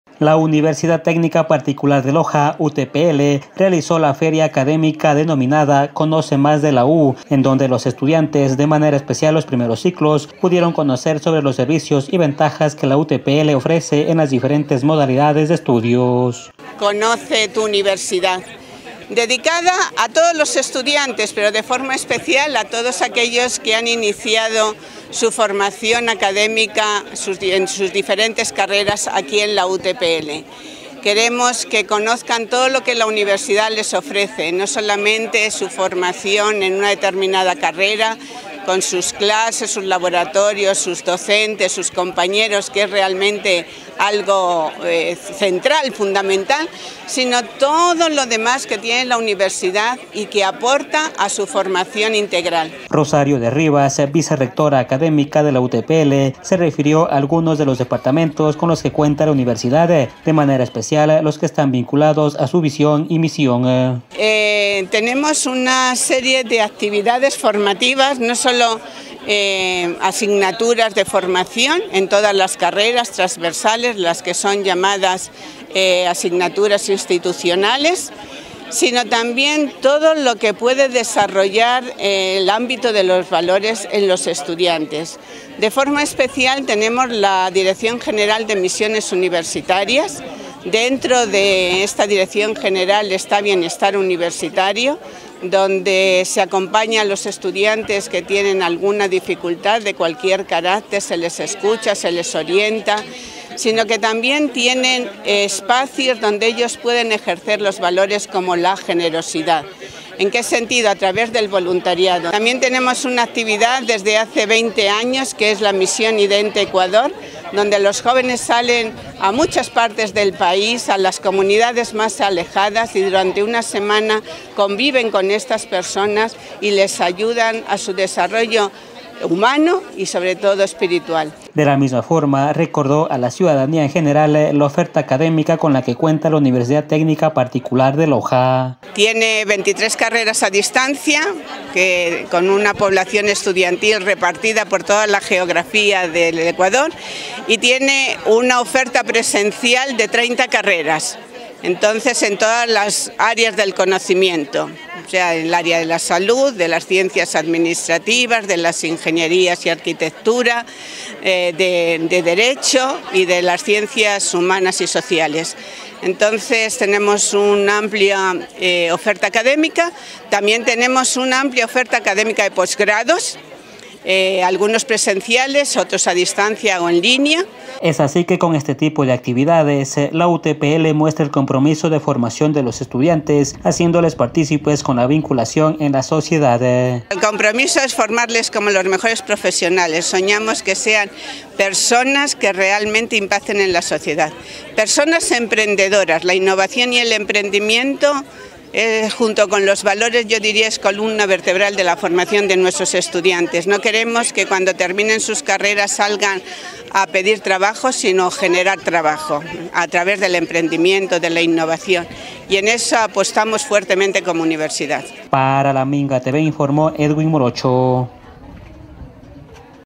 Feria Académica “Conoce más de la U”
La Universidad Técnica Particular de Loja (UTPL) realizó la Feria Académica “Conoce más de la U”, dirigida a los estudiantes para que conozcan sobre los servicios y ventajas que ofrece en sus modalidades de estudios.